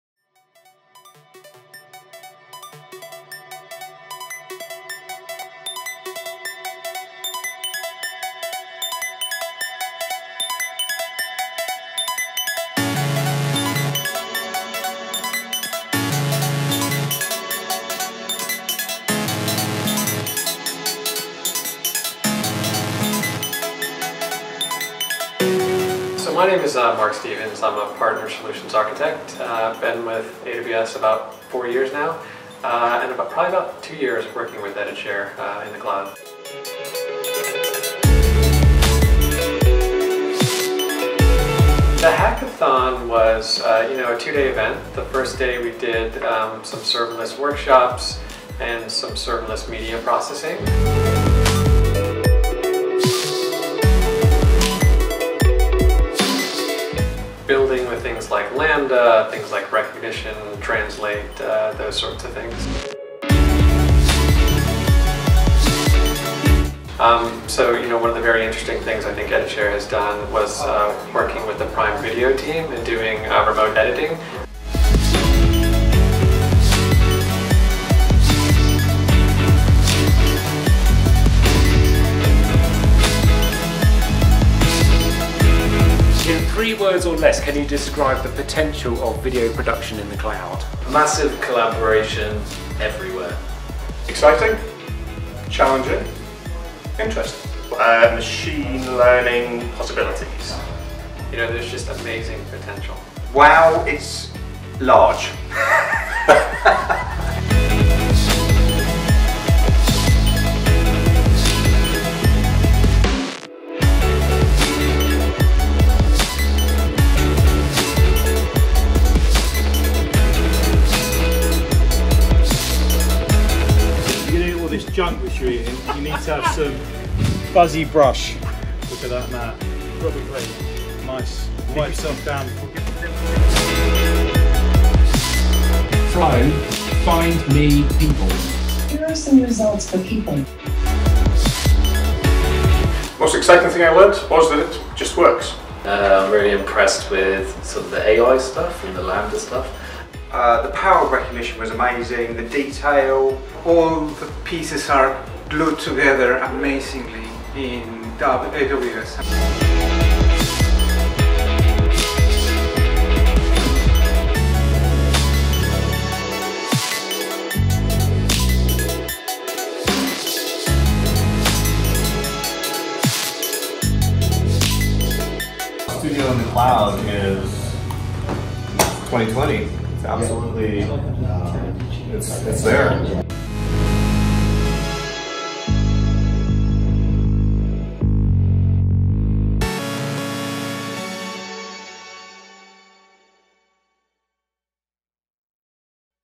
EditShare Cloud Hackathon
In March 2020, EditShare spent two days immersing in cloud technologies - collaborating with AWS to explore the next generation of cloud video production technologies.